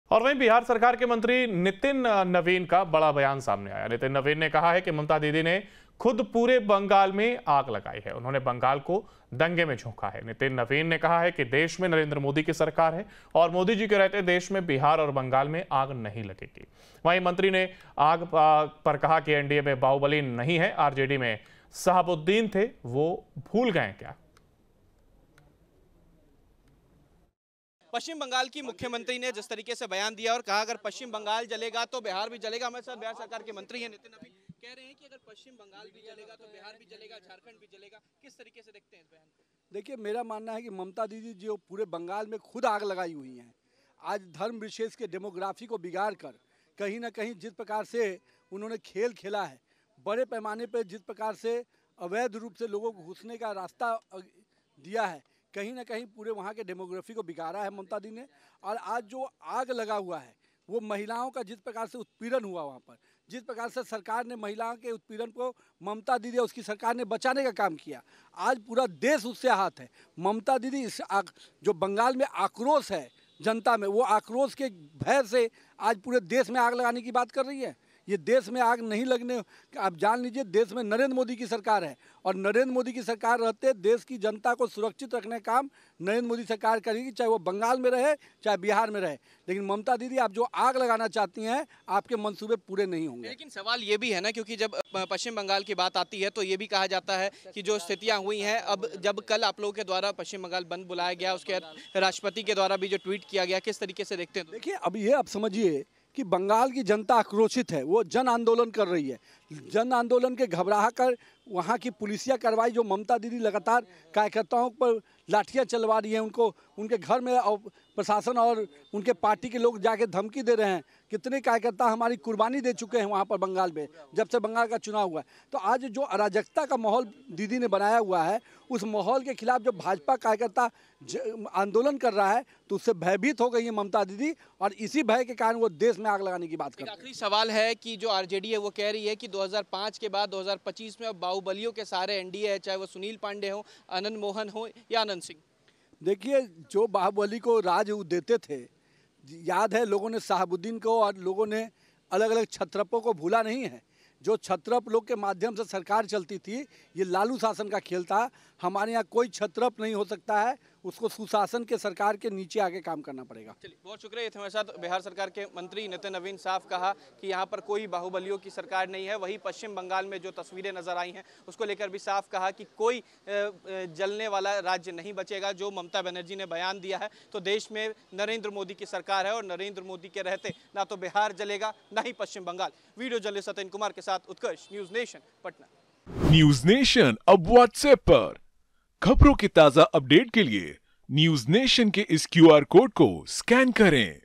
Jharkhand Politics : ममता बनर्जी पर भड़के मंत्री नीतिन नवीन, कहा 'दीदी ने खुद ही लगा ली आग'
Airtel - 378
TATA Play - 1131
DARSH - 525
GTPL -268